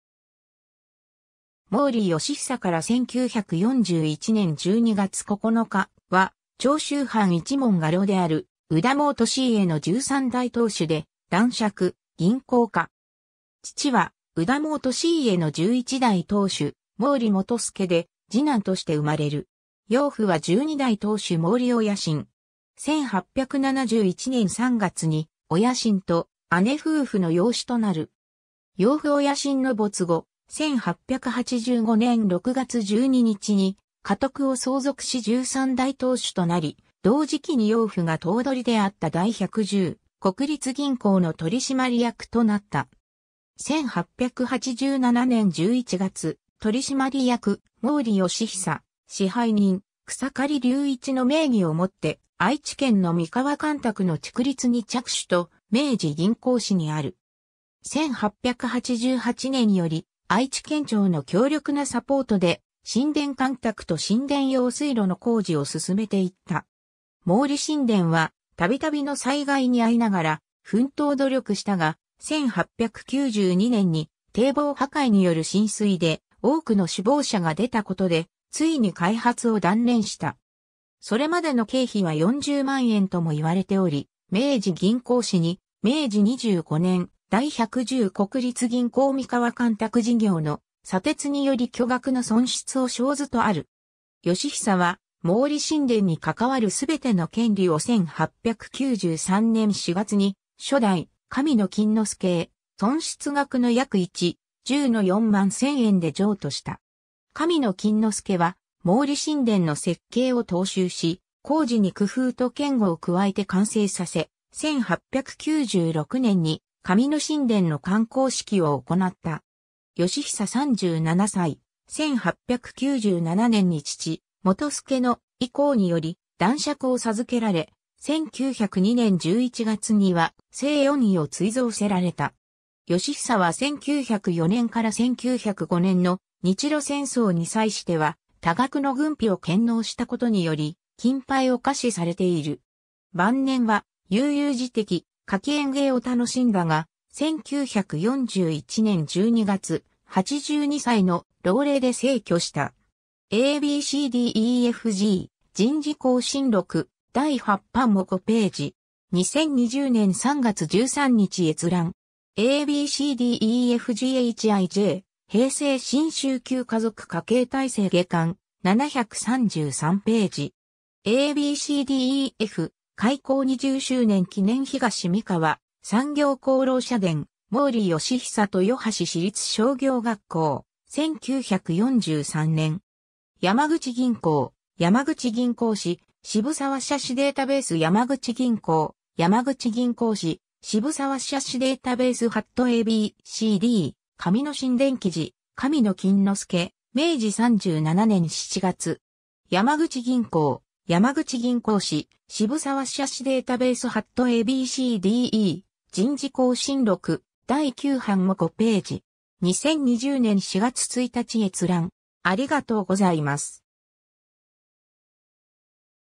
毛利祥久
毛利 祥久（もうり よしひさ、1860年4月8日（万延元年3月18日） - 1941年（昭和16年）12月9日）は、長州藩一門家老である右田毛利家の13代当主で、男爵、銀行家。
 父は右田毛利家の11代当主毛利元亮で、二男として生まれる。
養父は12代当主毛利親信。
1871年（明治4年）3月に親信と姉夫婦の養子となる。
養父親信の没後、1885年（明治18年）6月12日に家督を相続し13代当主となり、同時期に養父が頭取であった第百十国立銀行（後の山口銀行）の取締役となった（祥久が頭取だったとの情報もあるが、山口銀行史には取締役としか出てこない）。
 1887年（明治20年）11月、取締役毛利祥久、支配人草刈隆一の名義をもって愛知県の三河干拓（毛利新田）の築立に着手と明治銀行史にある。
 1888年（明治21年）より、愛知県庁の強力なサポートで新田干拓と新田用水路の工事を進めて行った。
 毛利新田は度々の災害に遭いながら奮闘努力したが、1892年（明治25年）に堤防破壊による浸水で多くの死亡者が出たことで、遂に開発を断念した。
 それまでの経費は40万円とも言われており、明治銀行史に「明治25年、第百十国立銀行三河干拓事業の蹉跌により巨額の損失を生ず」とある。
 祥久は毛利新田に係わる全ての権利を1893年（明治26年）4月に初代神野金之助へ、損失額の約1/10の41,000円で譲渡した。
 神野金之助は毛利新田の設計を踏襲し工事に工夫と堅固を加えて完成させ、1896年（明治29年）に神野新田の完工式を行った。
 祥久37歳、1897年（明治30年）に父元亮の遺功により男爵を授けられ、1902年（明治35年）11月には正四位を追贈せられた。
 祥久は1904年（明治37年）から1905年（明治38年）の日露戦争に際しては、多額の軍費を献納した事により金盃を下賜されている。
 晩年は悠々自適、花卉園藝を楽しんだが、1941年（昭和16年）12月、82歳の老齢で逝去した。
 ^ a b c d e f g 『人事興信録 第8版』モ5頁（国立国会図書館デジタルコレクション）。
2020年3月13日閲覧。
 ^ a b c d e f g h i j 『平成新修旧華族家系大成』下巻、733頁。
 ^ a b c d e f 『開校廿周年記念東三河産業功労者伝『毛利祥久』』豊橋市立商業学校、1943年。
 ^ (株)山口銀行『山口銀行史』(1968.09) 渋沢社史データベース ^ (株)山口銀行『山口銀行史』(1968.09) 渋沢社史データベース ^ a b c d 『神野新田紀事』神野金之助、明37年7月（1904）。
 ^ (株)山口銀行『山口銀行史』(1968.09) 渋沢社史データベース ^ a b c d e 『人事興信録 第9版』モ5頁（国立国会図書館デジタルコレクション）。
2020年4月1日閲覧。